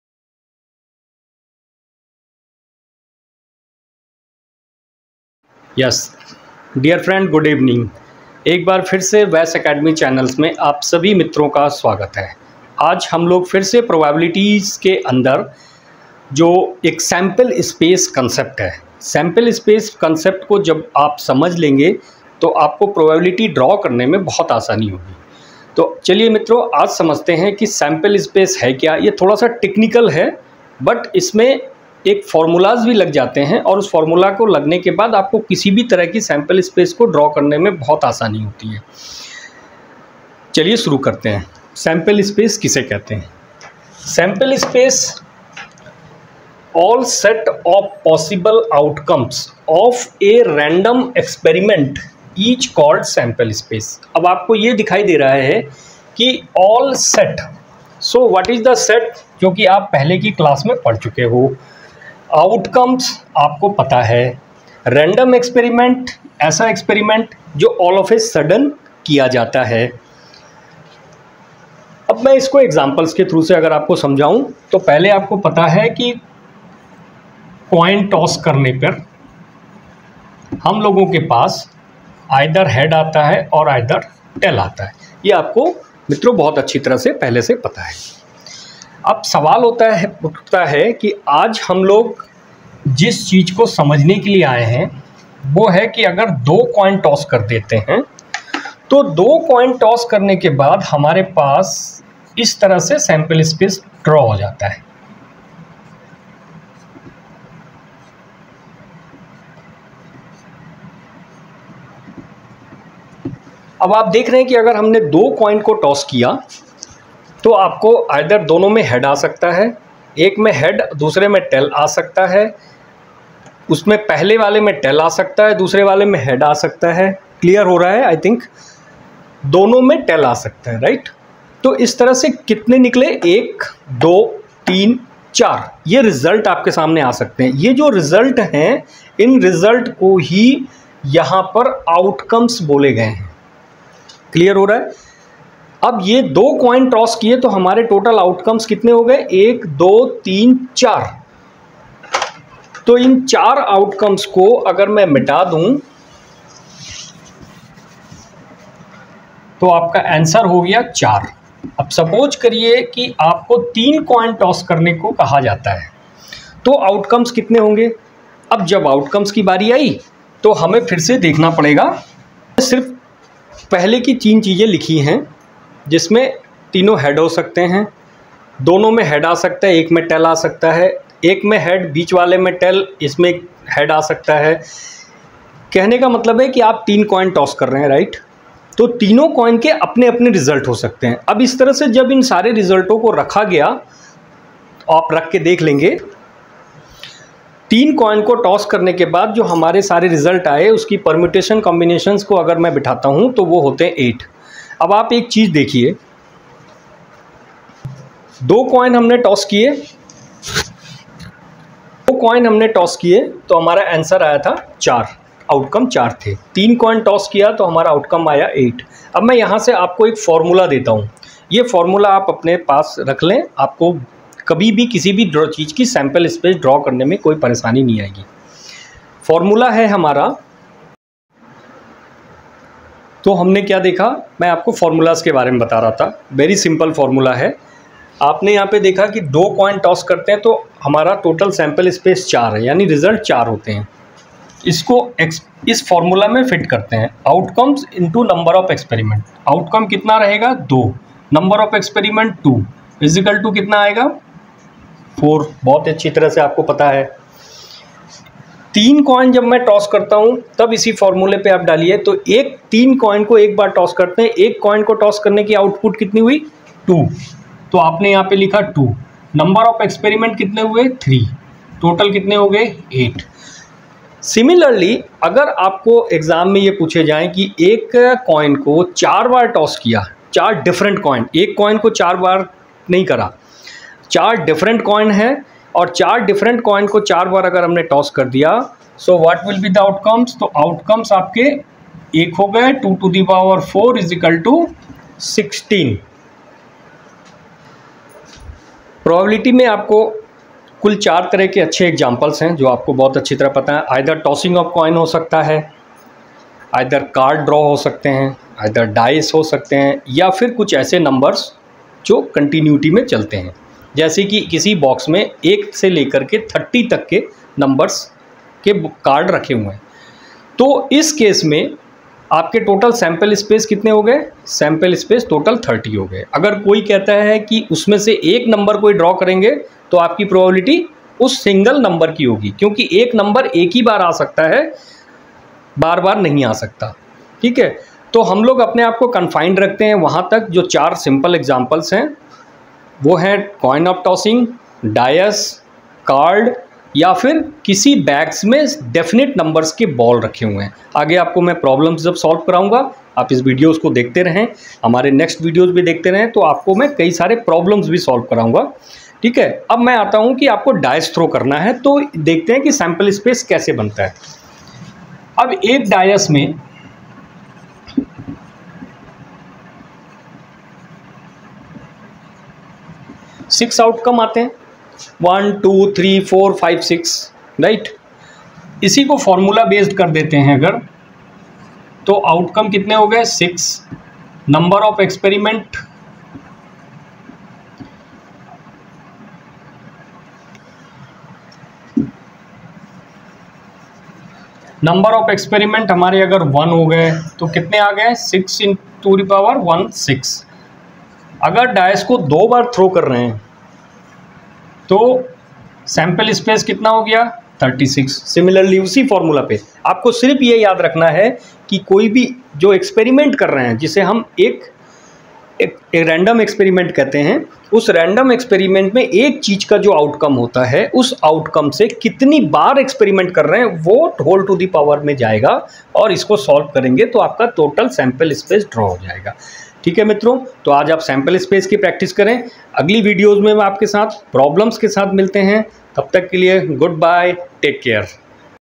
sample space probability/for class IX, X, XI, XII, CA, CMA, CS, MBA, QUANTITATIVE MATH
This video is about to learn SAMPLE SPACE in probability. Sample space is the counting of all the possible set of outcomes in a random experiment. The knowledge would help the viewers to solve the assignment of probability, where the denominator will always represent the sample space.